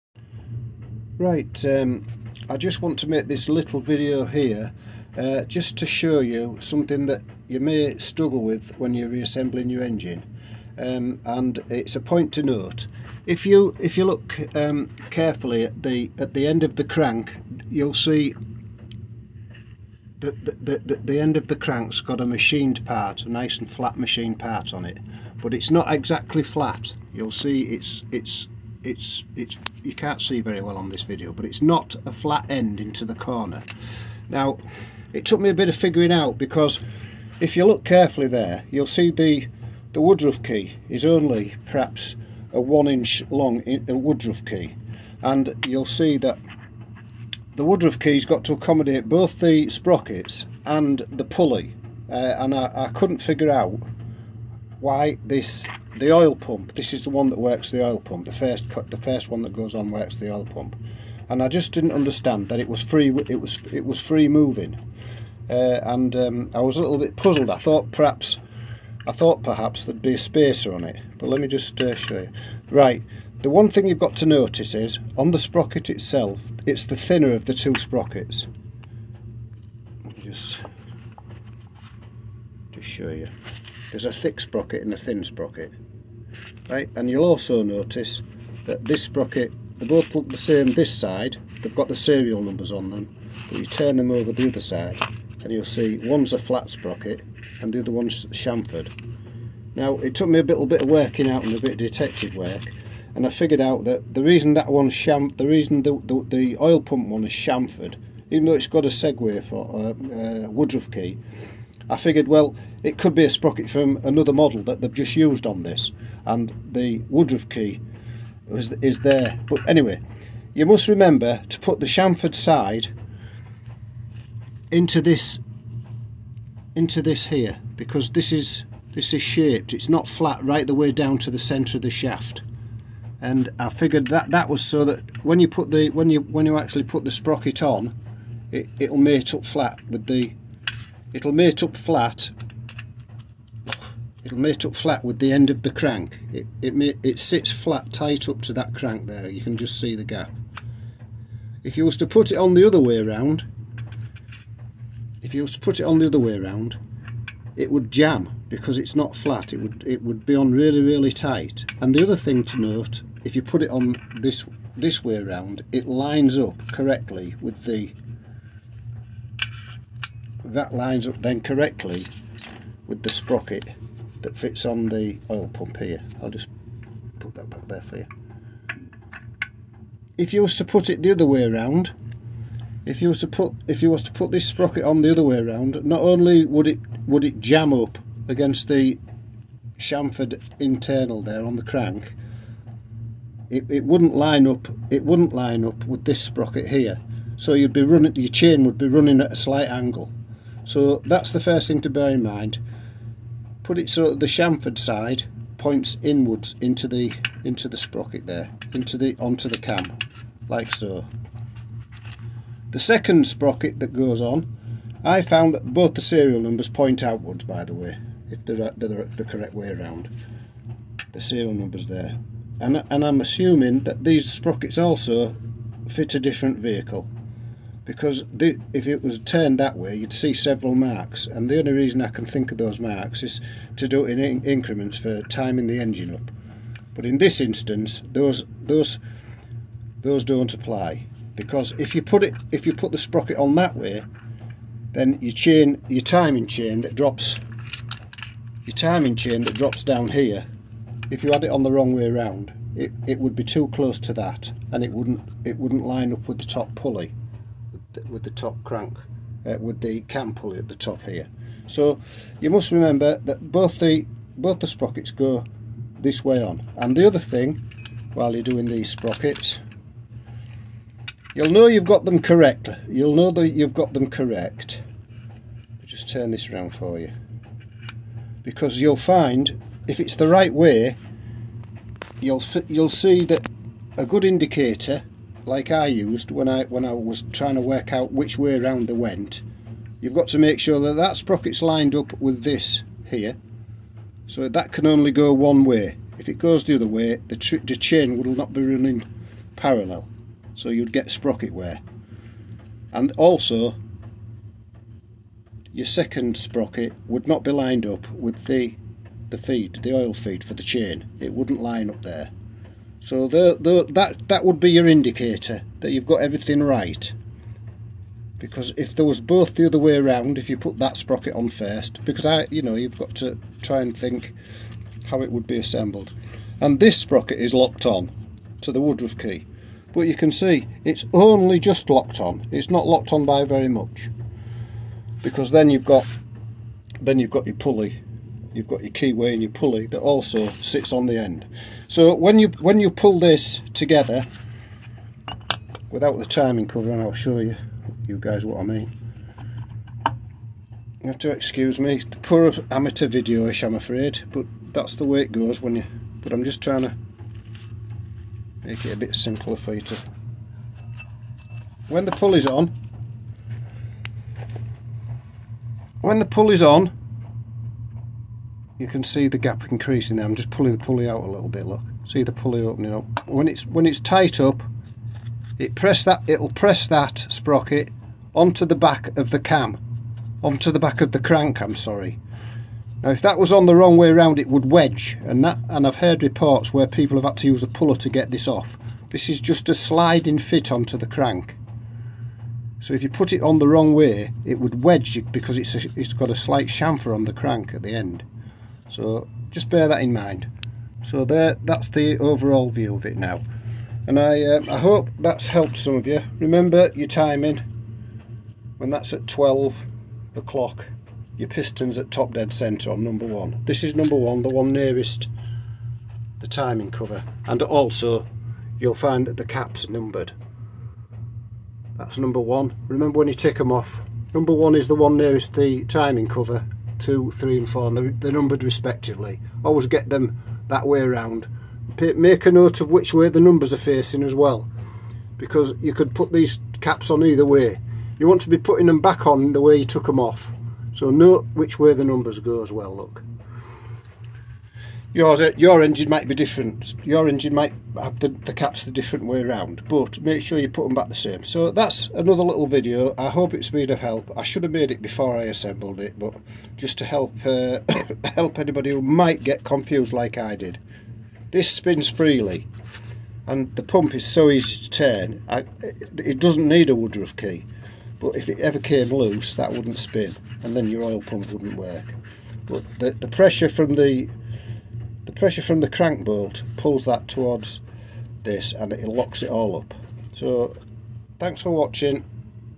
SMART FORTWO 450 TIMING SPROCKETS.
fitting oil pump and timing sprockets